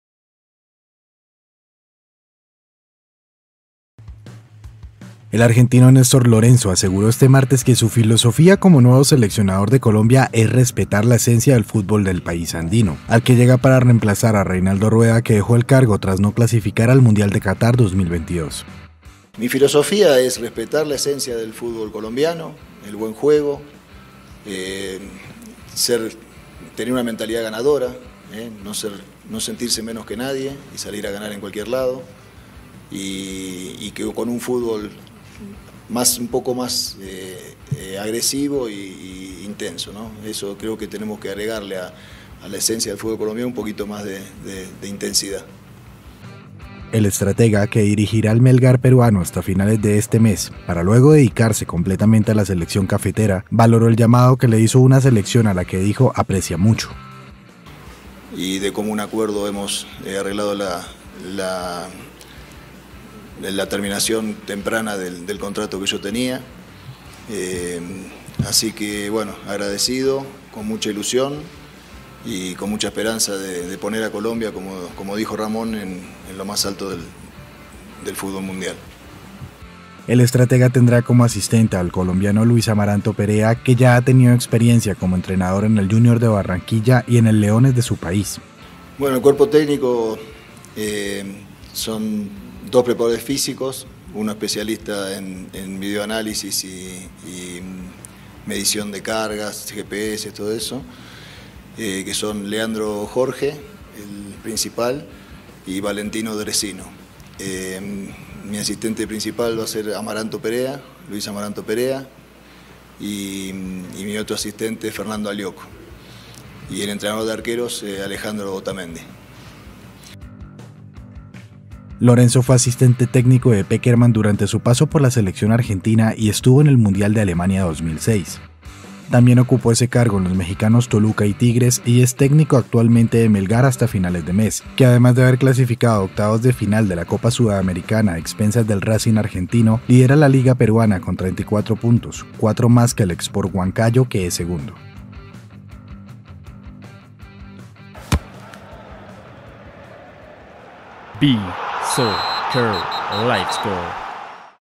Néstor Lorenzo es presentado como el nuevo director técnico de la Selección Colombiana de Fútbol
El argentino Néstor Lorenzo aseguró este martes que su "filosofía" como nuevo seleccionador de Colombia es "respetar la esencia" del fútbol del país andino, al que llega para reemplazar a Reinaldo Rueda que dejó el cargo tras no clasificar al Mundial de Catar 2022.